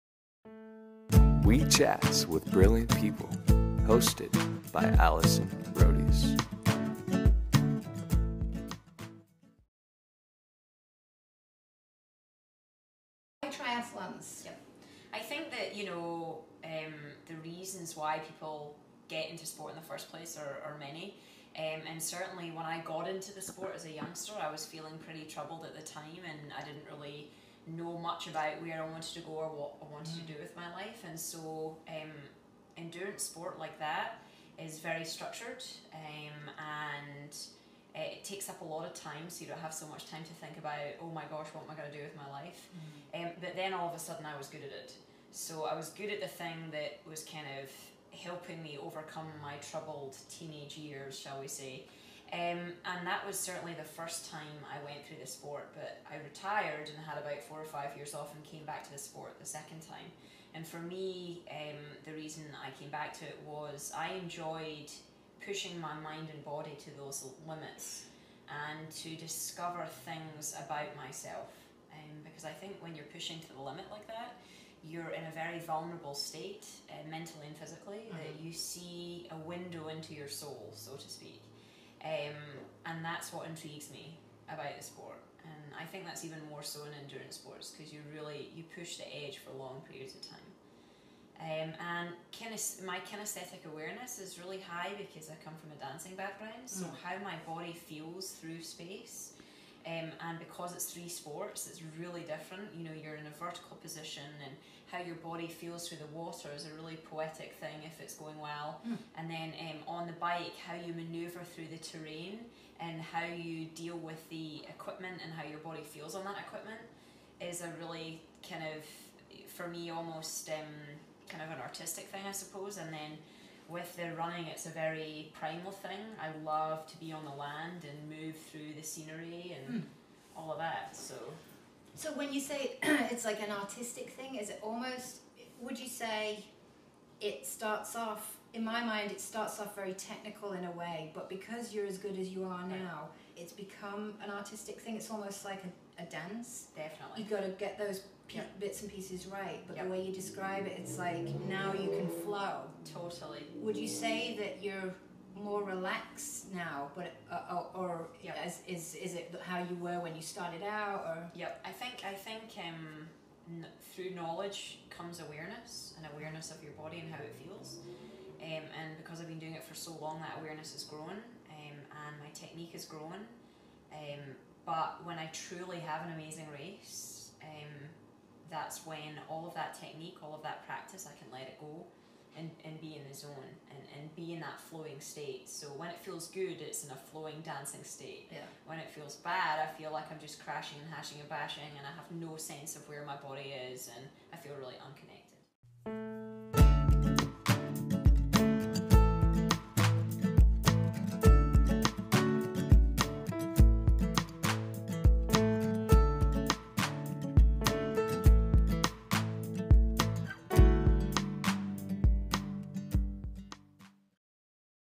Why Lesley Paterson Does Triathlons And Her Kinaesthetic Awareness Experience | Wee Chats
In the second part of this Wee Chat, Lesley Paterson explains why she became a triathlete and how it helped overcome her trouble teenage life. She also shares about her kinaesthetic awareness experience as a triathlete.

This interview was recorded on November 26th, 2011